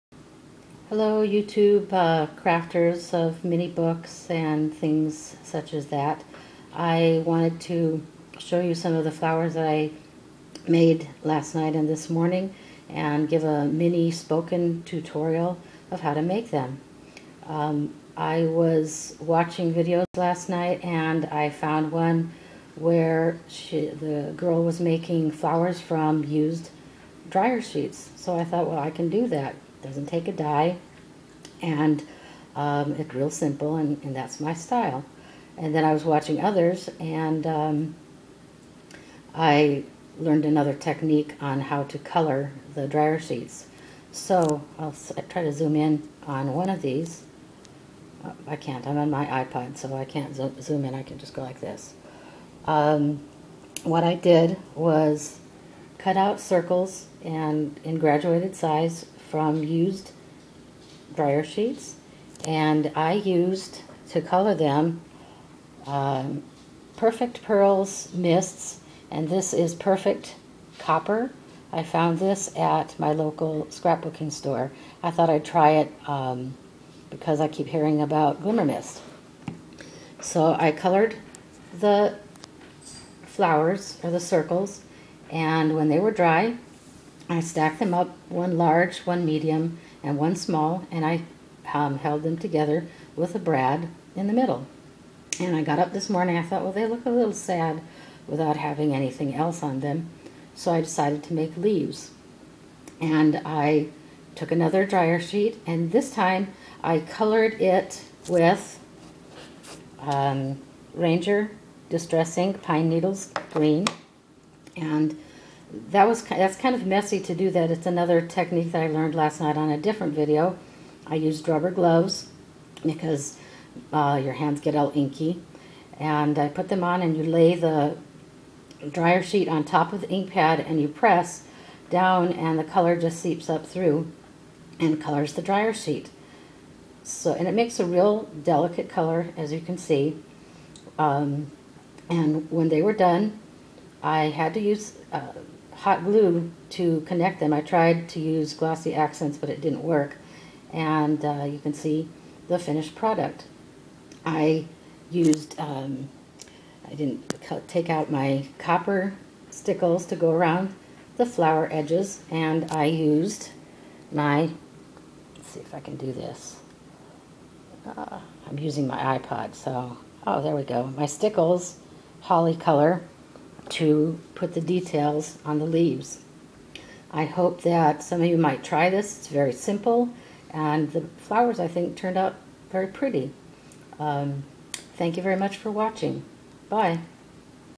Used Dryer Sheet Flowers - Tutorial
I can't take credit for these flowers, I used three different techniques I learned on YT videos that I watched last night.
I hope the quality of this video is ok, as I filmed it with my iPod!
Here are the links where I got the ideas/tecniques that I combined to make them.
I am looking for the rest, please be patient
I can't find the other videos I watched, I apologize to those YouTubers who made them! In my search though, I found several other videos on making recycled dry sheet flowers, just search for them. Check them out for different ideas and techniques.